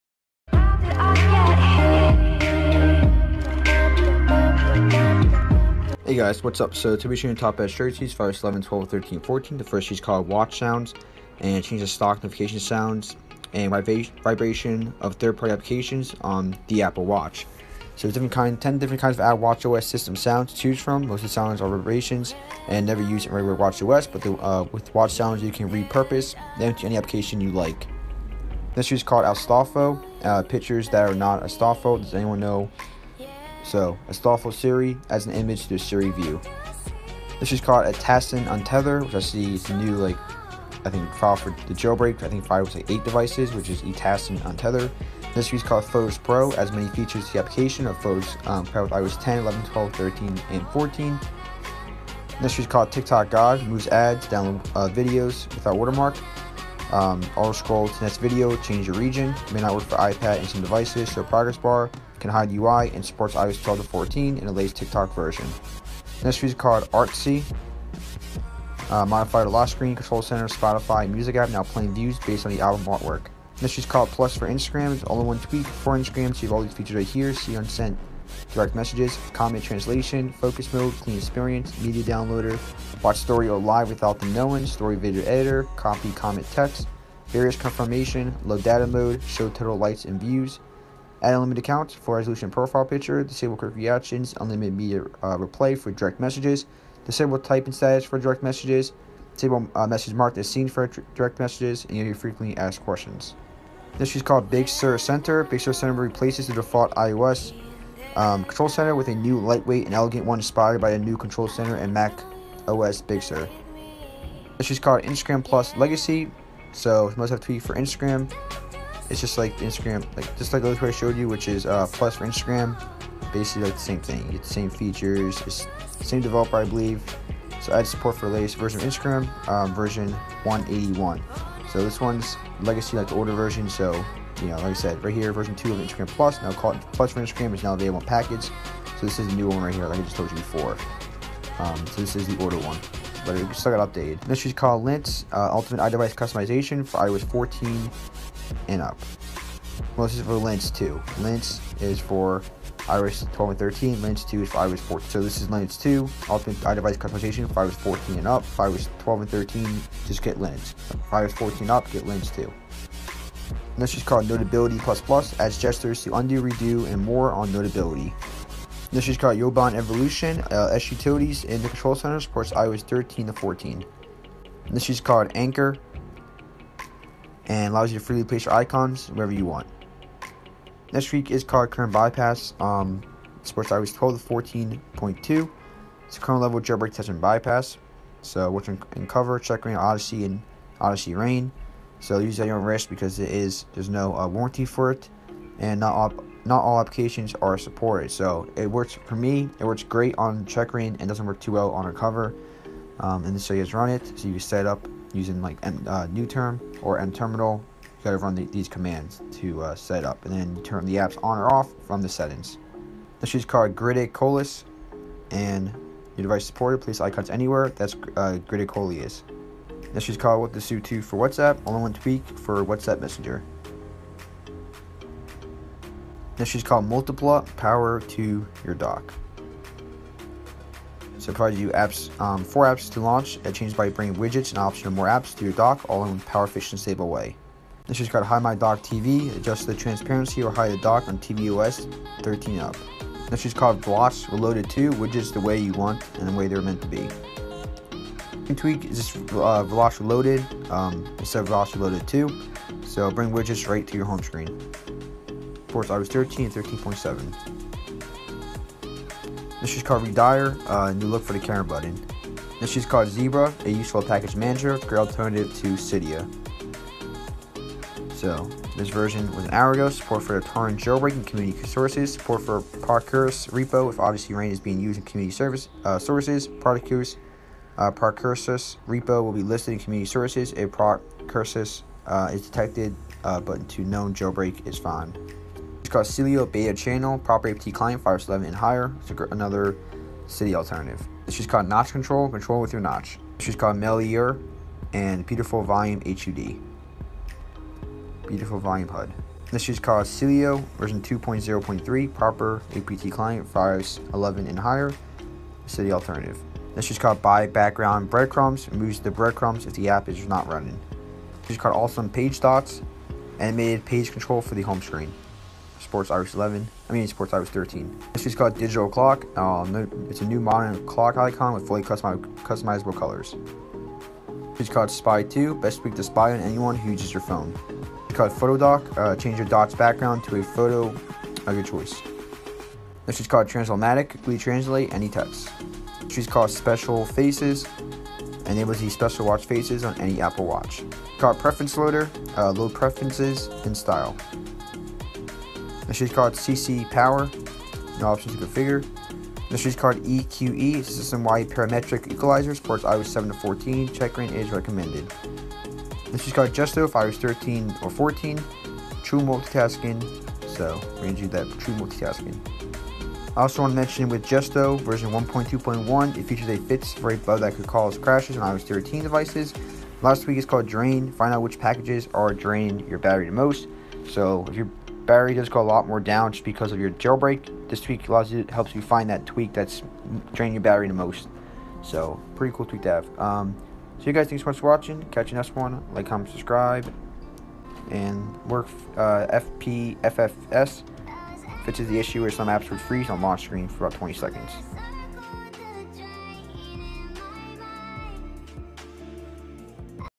Top BEST Updated Jailbreak Tweaks for iOS 11/12/13/14 (Checkra1n, Odyssey, Unc0ver + Chimera)
In this video I show you my top & best updated iOS 11 / 12 / 13 / 14 jailbreak tweaks compatible with the Checkra1n, Odyssey, Chimera and Unc0ver jailbreaks. Comment down any questions or concerns you may have. Peace!